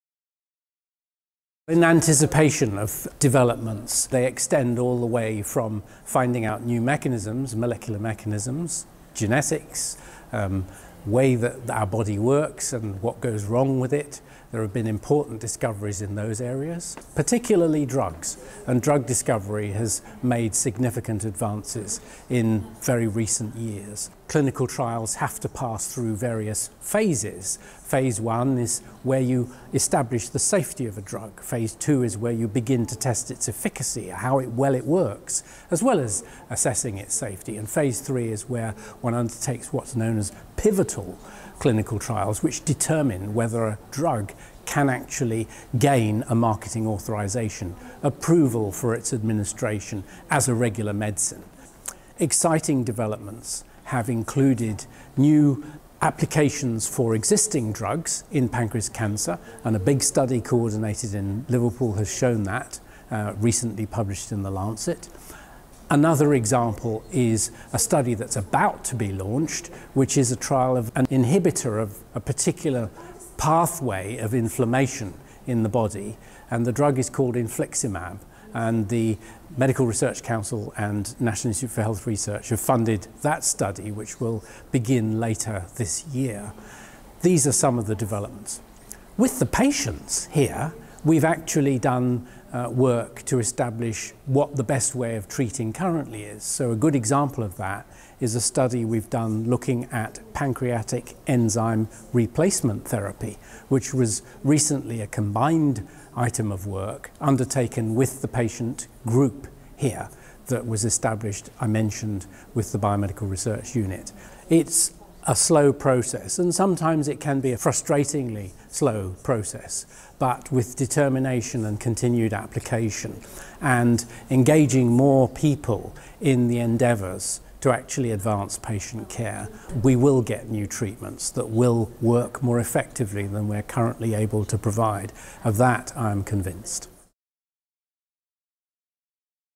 Professor Sutton on clinical research into pancreatic treatments:
Professor Robert Sutton talks about research into pancreatic conditions and the effects trials will have on future patients.